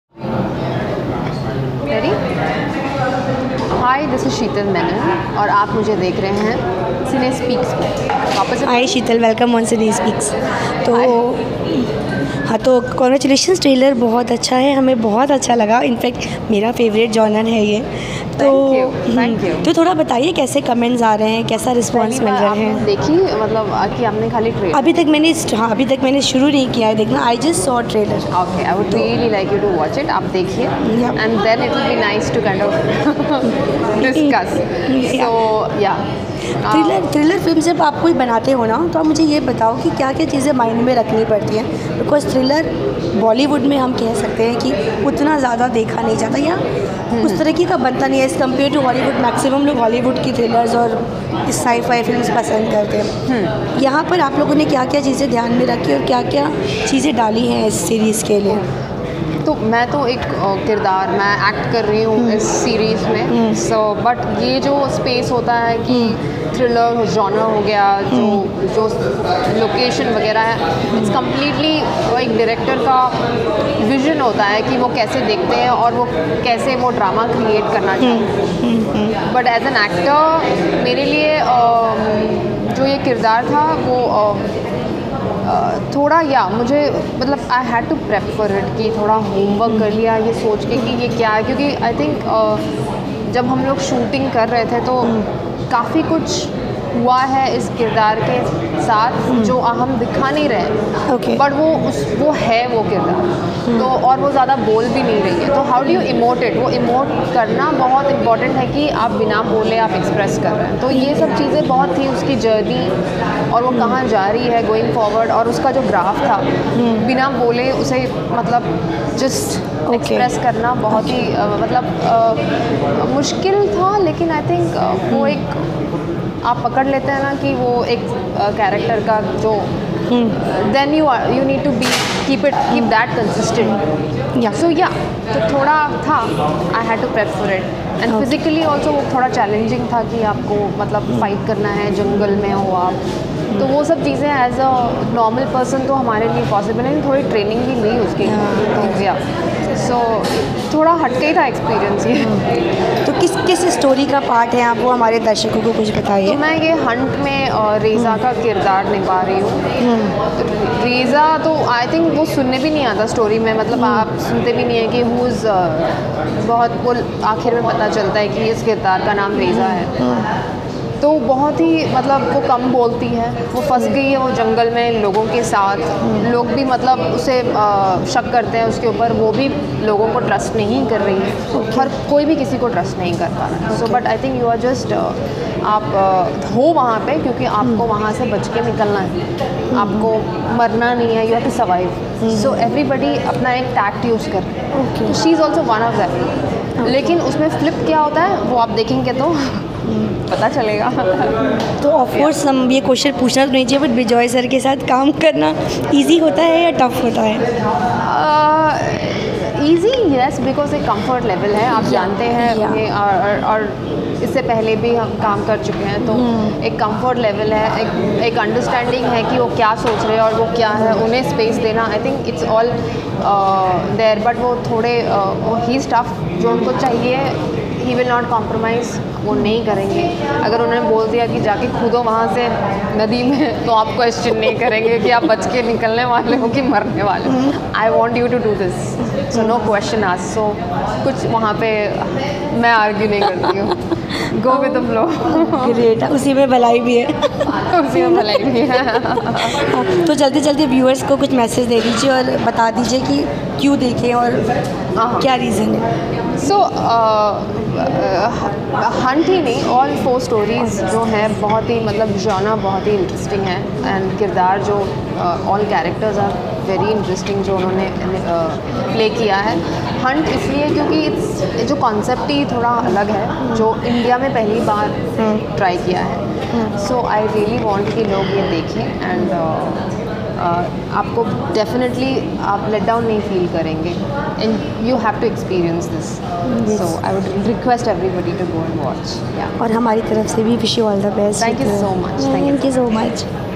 Sheetal Menon Exclusive n Candid with Cinespeaks for new Webseries Flip..
SheetalMenon Exclusive with Cinespeaks for Flip ..